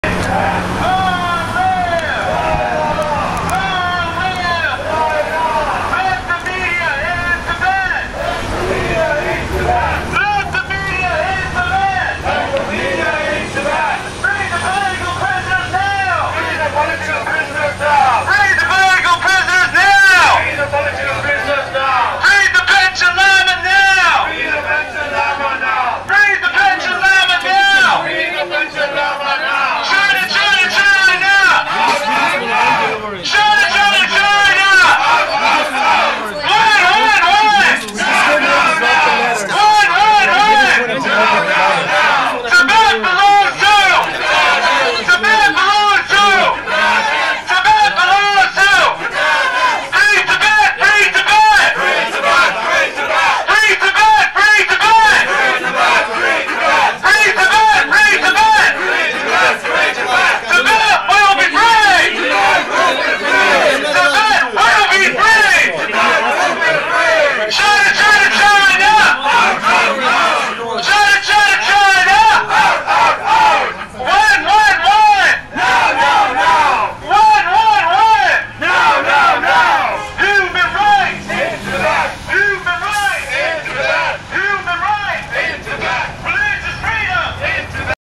FREE DHONDUP WANGCHEN NOW!
(San Francisco, CA)  Lhamo Tso, wife of Tibetan film maker and Prisoner of Conscience Dhondup Wangchen leads a small delegation of Tibetan community leaders and Amnesty USA Activists in an attempt to hand deliver hundreds of letters and petitions calling for the release of her husband from the Chinese government.